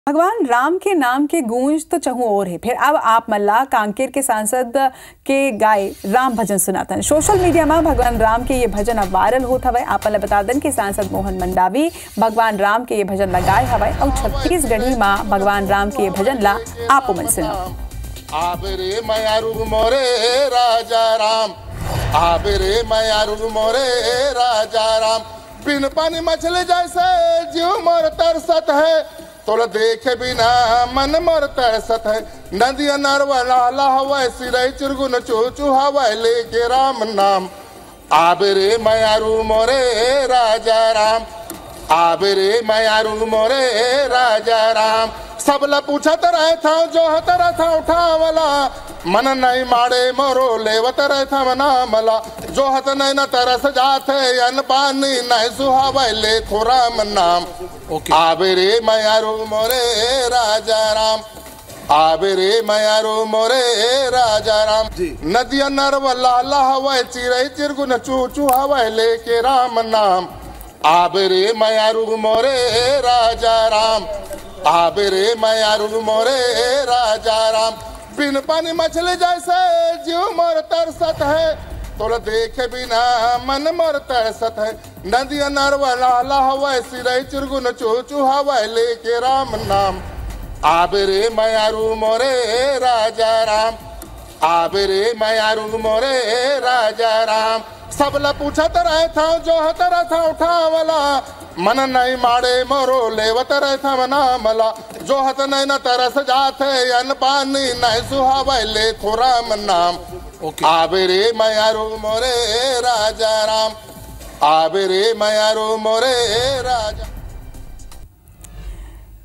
संसद भवन म भांचा राम के भजन के गूंज | कांकेर के सांसद Mohan Mandavi गाइन भजन | देखव वीडियो
IBC24, संसद भवन म भांचा राम के भजन के गूंज, कांकेर के सांसद Mohan Mandavi गाइन भजन, देखव वीडियो